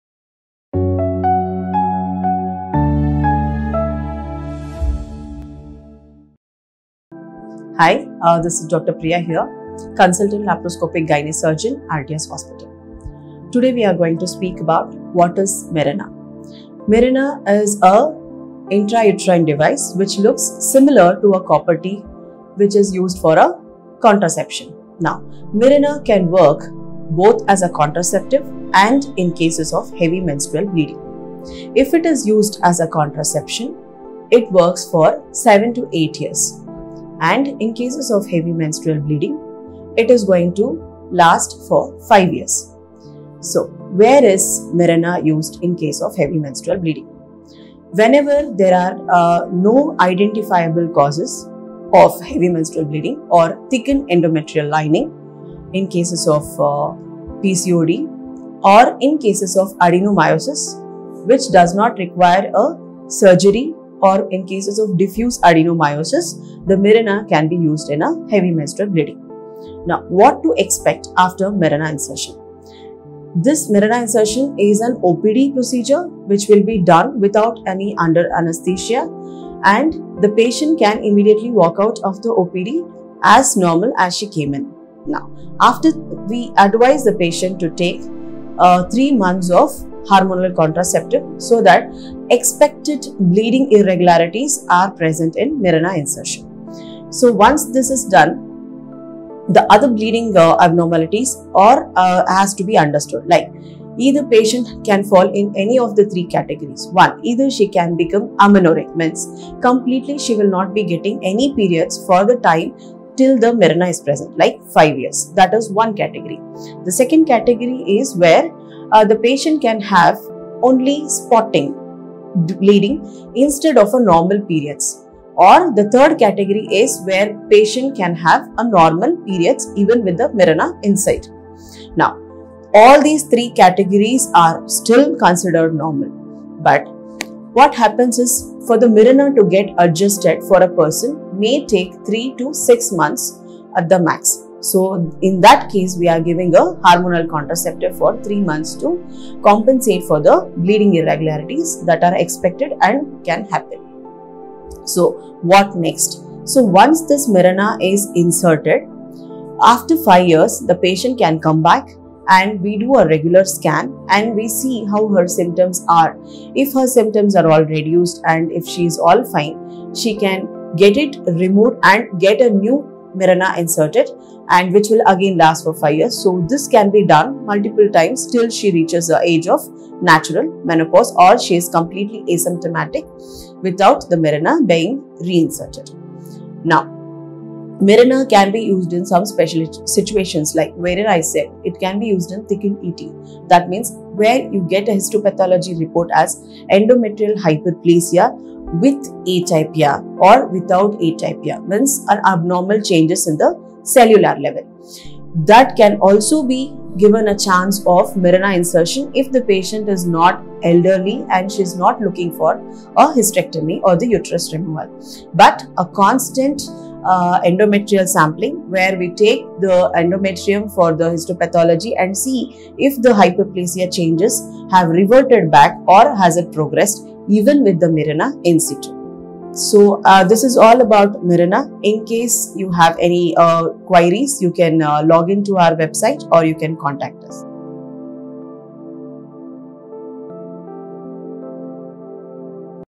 What Mirena (IUD Insertion)? Is it an Effective Contraceptive Method?
🎥 Check out this informative video featuring Dr. Priya S. who's a consultant-Gynaec  Laparoscopic Surgeon from Little Feet by Altius Hospitals, as she discusses the benefits of intrauterine devices (IUDs). Discover how IUDs provide a highly effective and convenient contraceptive option for women. Learn about their long-lasting protection, easy insertion process, and low-maintenance nature. Don't miss out on this valuable insight into a reliable birth control method!